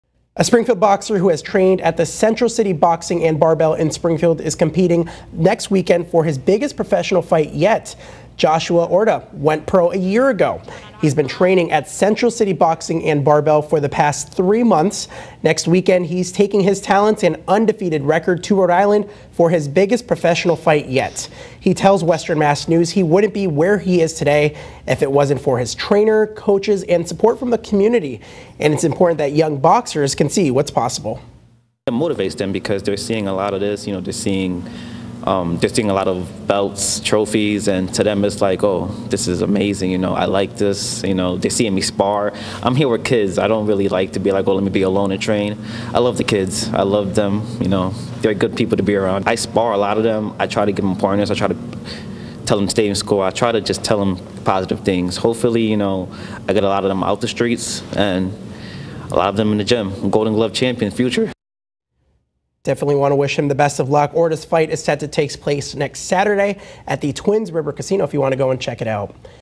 Pro boxer inspiring western Mass youths to achieve their dreams
A Springfield man is putting the city and a local boxing gym on the map.